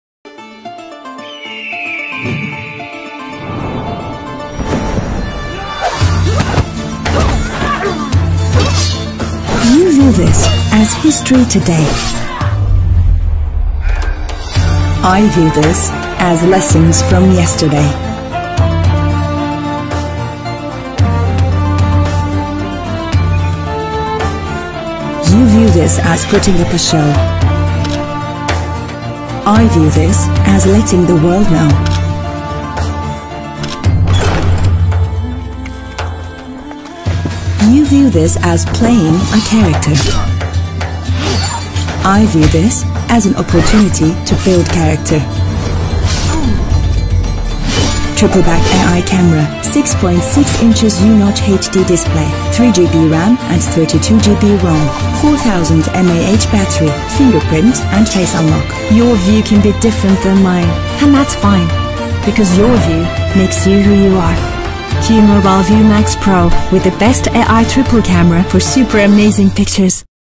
Halema Sultan in Pakistani Ad - Esra Bilgiç in Pakistani TV AD
Halema Sultan  (Esra Bilgiç) in Pakistani TV advertisement for the first time.